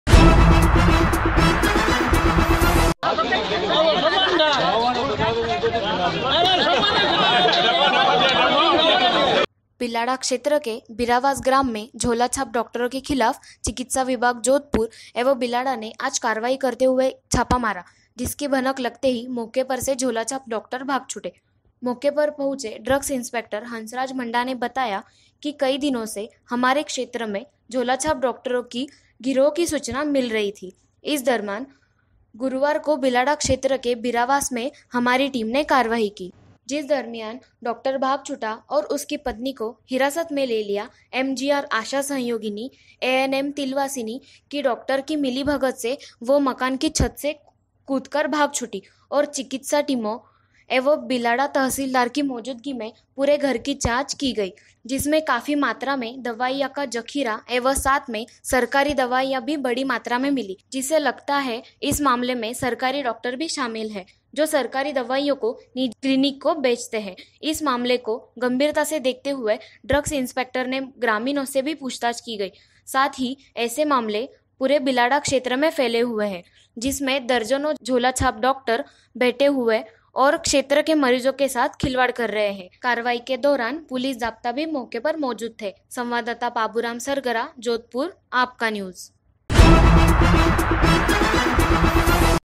AAPKA NEWS - झोला छाप डॉक्टरों के खिलाफ मारा छापा
मरीजो के साथ डॉक्टर ने की  खिलवाड़
 सरकारी डॉक्टर भी शामिल,घर में मिली सरकारी दवाइयां
मौके पर पहुंचे पुलिस ने की कारवाई